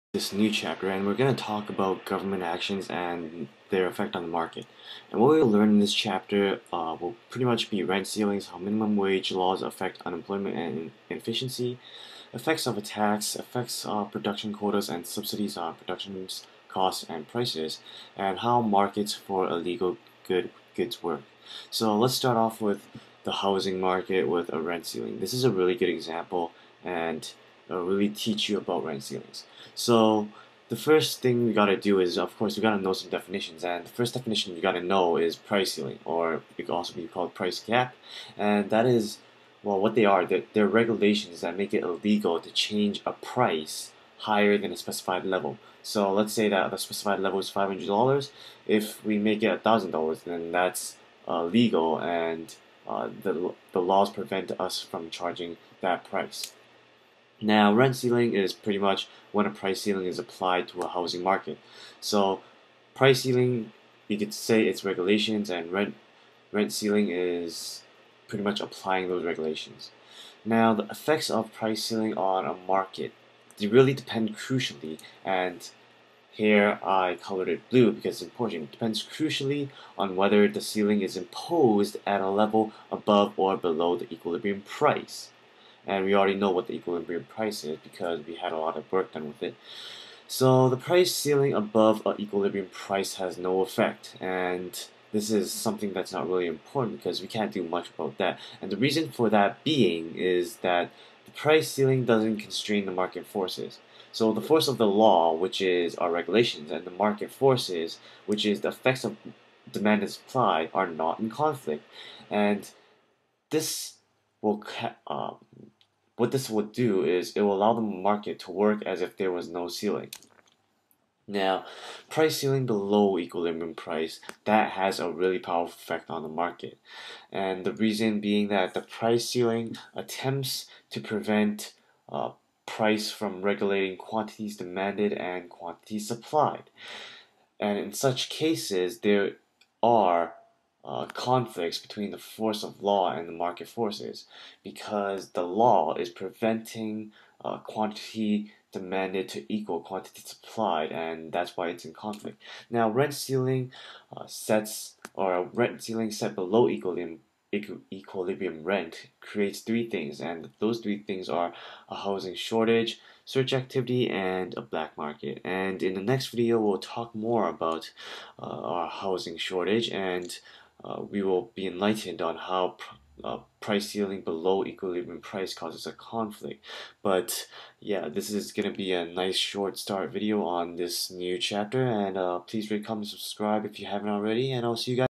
Microeconomics - 79: Housing Market & Rent Ceilings
A new chapter and new definitions. Again make sure you pay attention, i think this video is most important in terms of knowing the basics.
Must knows:
- Definitions
- Facts and reasons (i.e. the purple reason and the statement above it)